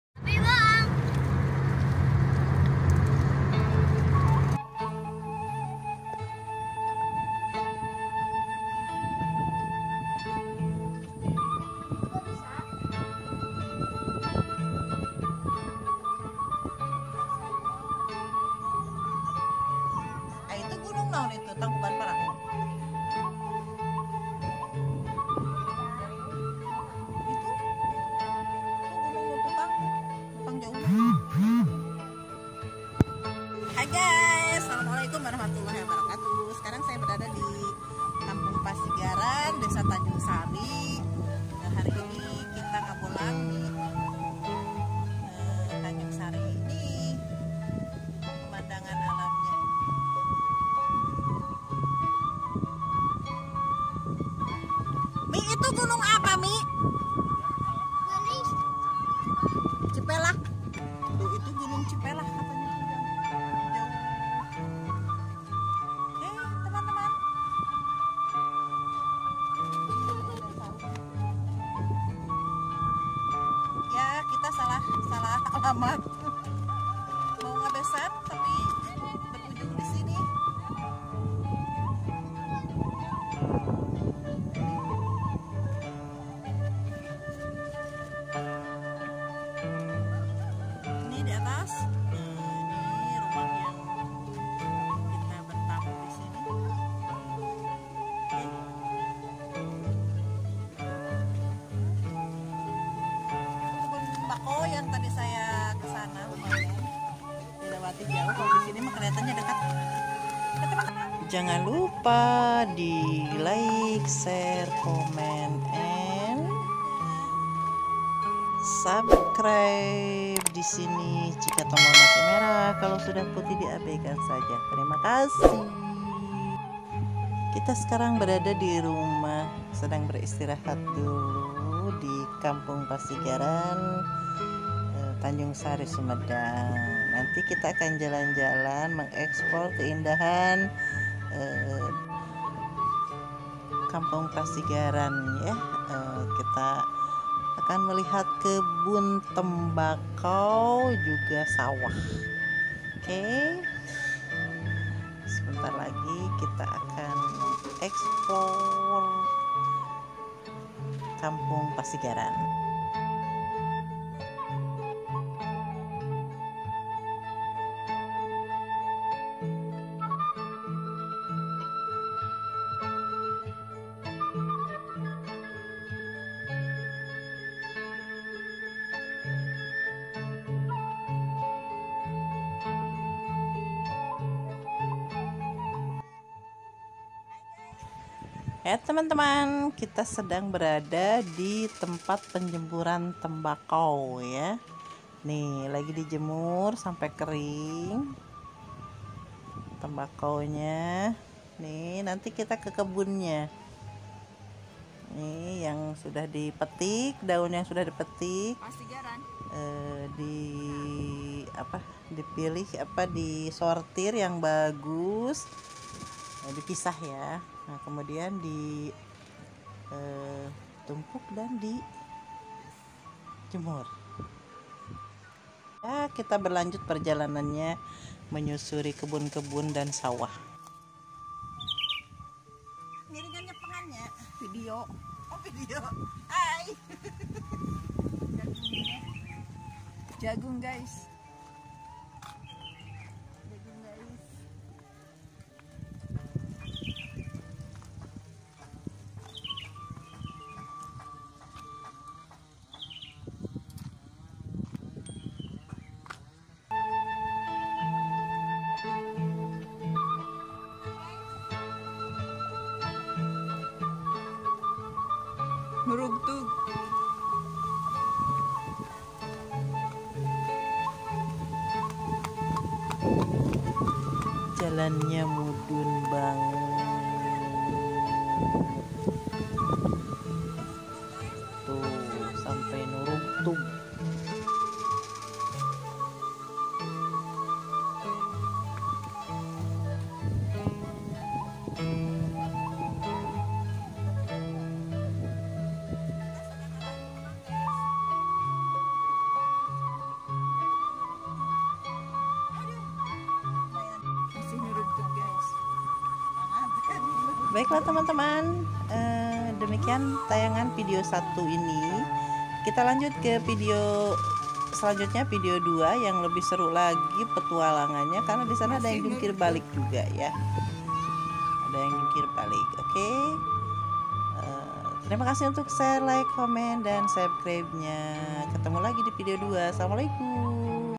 Me and My Student (ngabolang dan meliput proses pengeringan atau penjemuran bakau)
Ngabolang dan mengeksplore keindaha kampung pasigaran Tanjungsari Sumedang